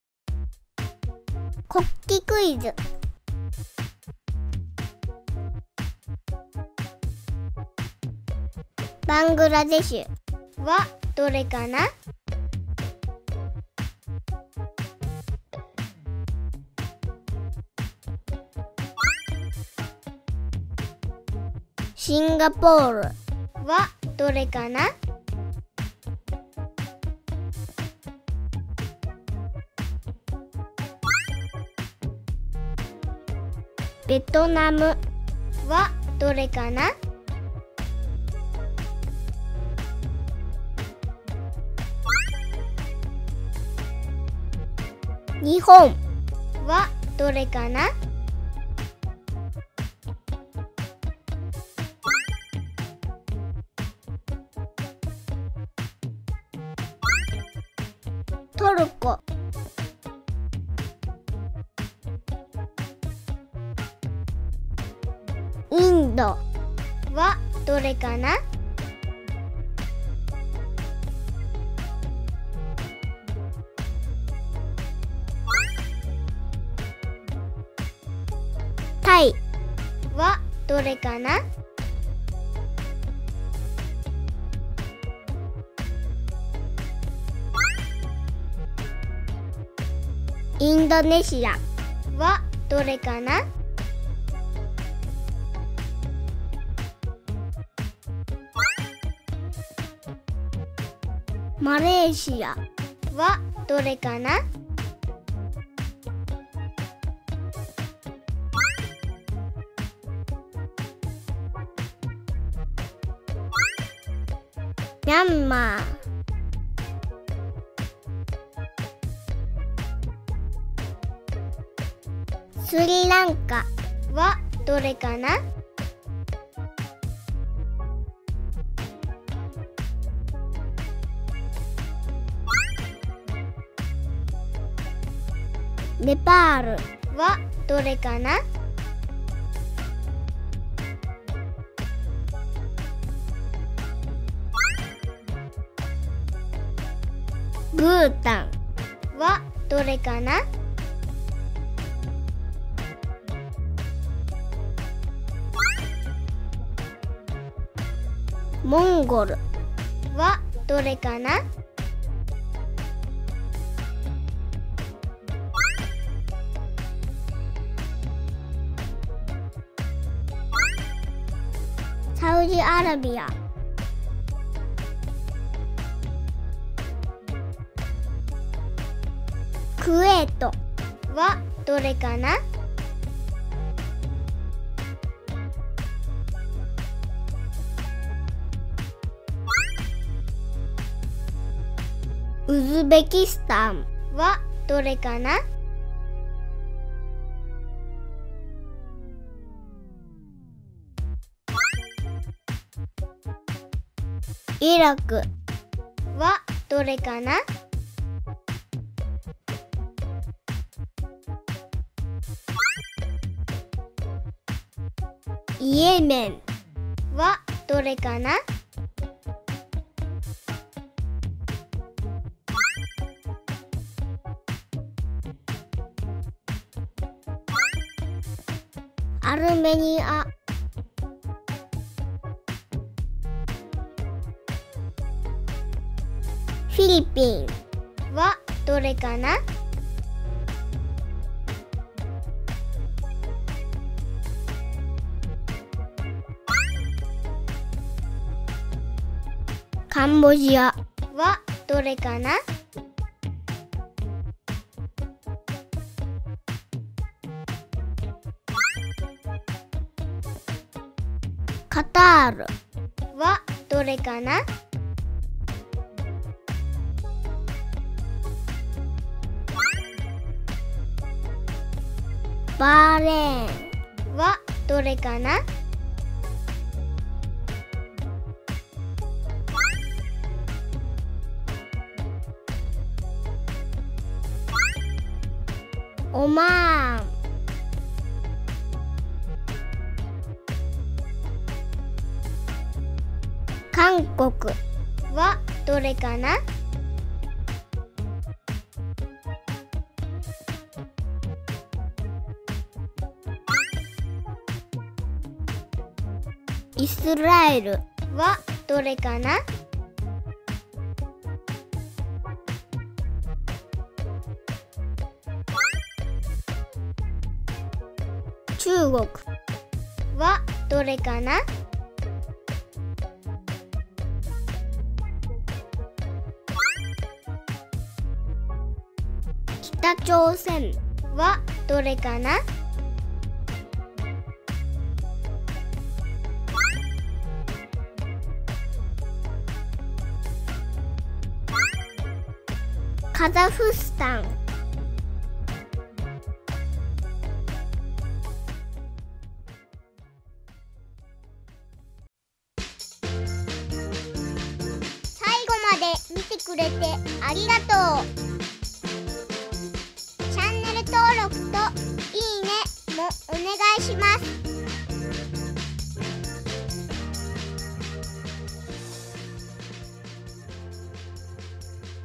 【国旗クイズ・アジア編】世界の国旗をおぼえよう！知育【赤ちゃん・子供向けアニメ】World flag quiz / Asia
クイズ形式でアジアの国々の国旗を覚える知育アニメです。

〜ノッカーナアニメーションは「いないいないばあ」アニメや赤ちゃんと子供たちの知育、教育にお役立ていただけるような楽しく優しい動画作りを心がけております〜

This is animation channel for baby and kids.